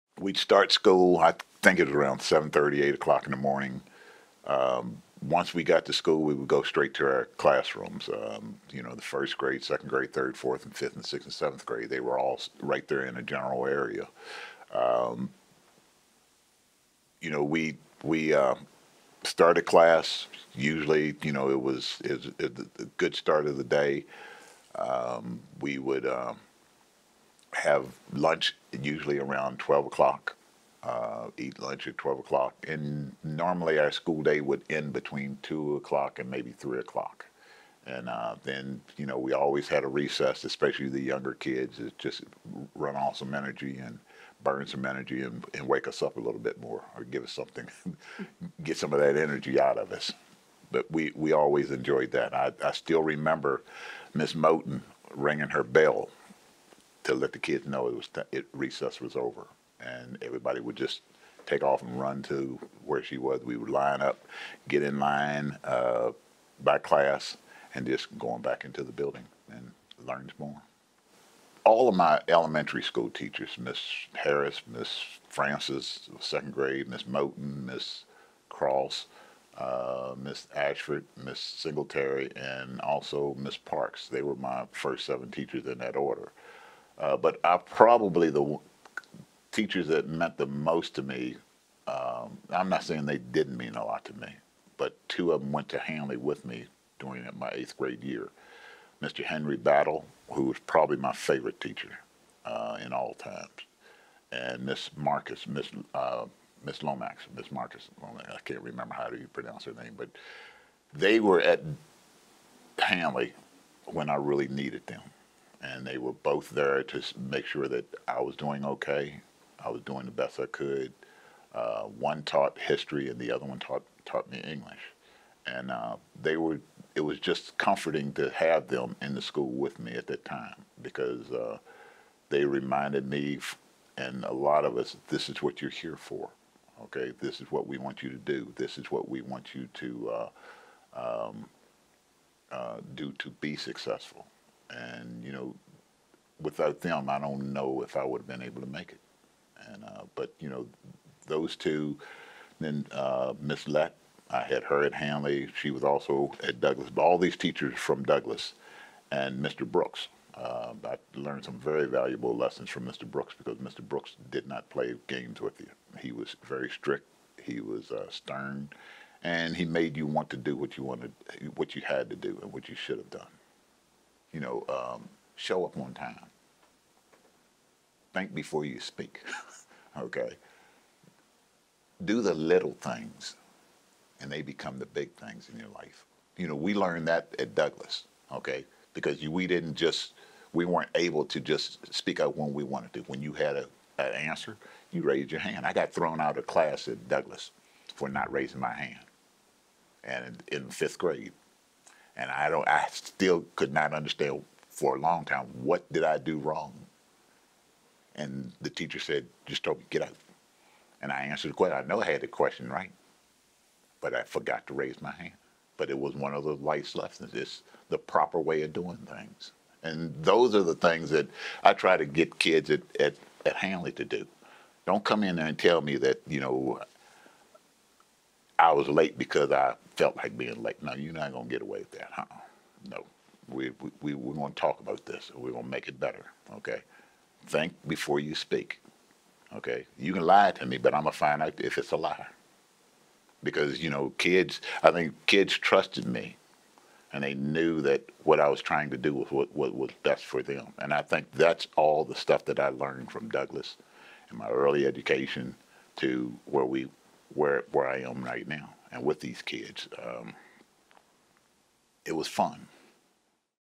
Tommy Dixon - Academics
This video series was created for the Douglas School Museum in Winchester, VA. The Douglas School is remembered as a beacon of Black excellence in Winchester and the surrounding areas during segregation (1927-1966).